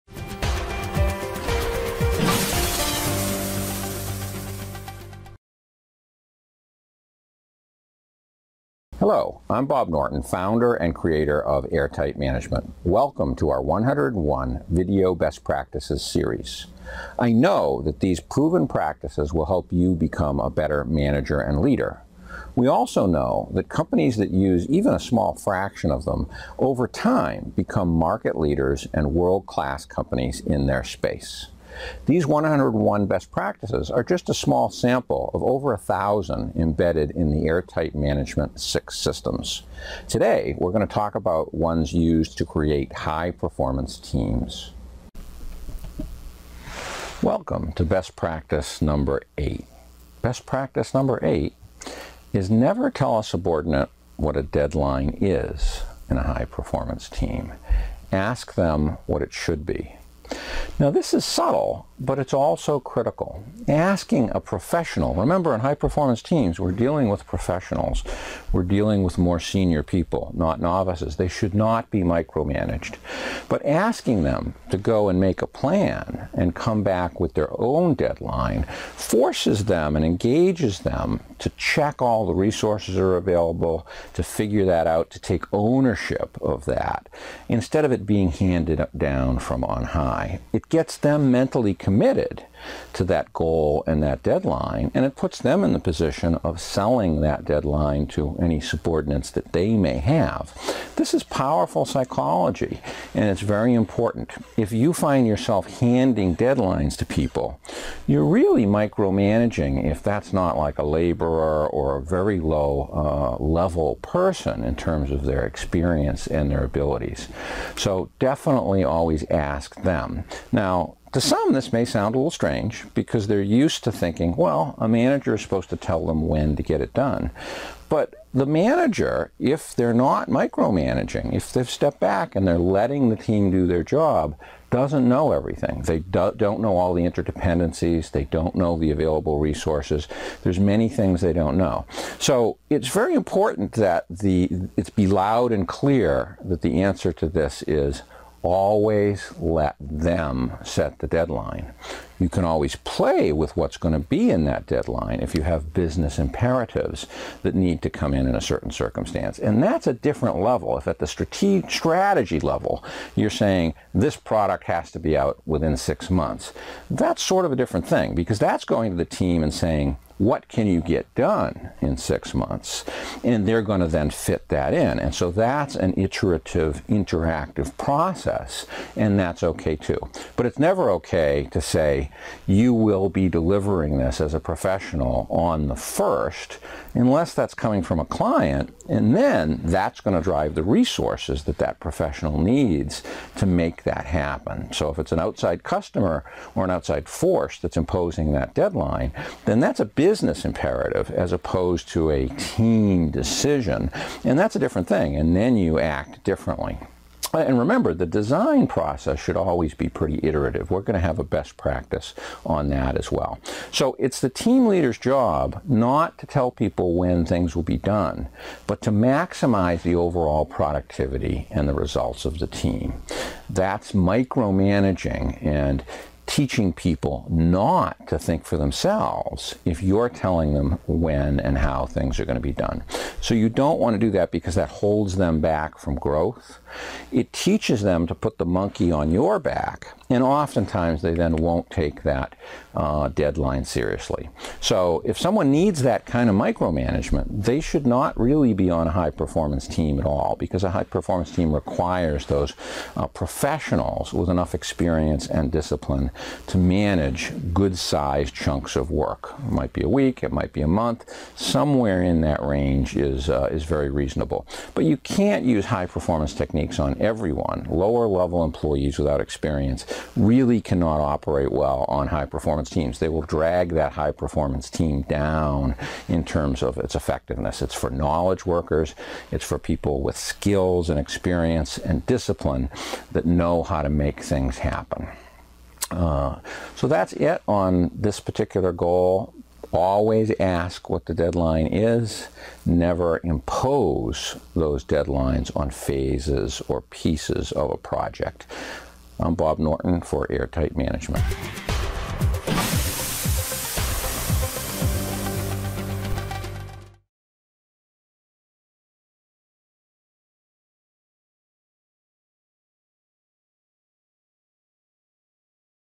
Best Practices Series High-Performance Teams #8 - Let people set their own deadlines
High-Performance Team Best Practice # 8 - Counter intuitively a boss should never tell an experienced team member when something is due. Then they are not committed, do not figure out the plan and do not feel empowered to make it happen sooner. This can degrade leadership and performance. A key principle of Management By Objective (MBO or OKR if you prefer) is letting people set their own deadlines. This video describes why this is true.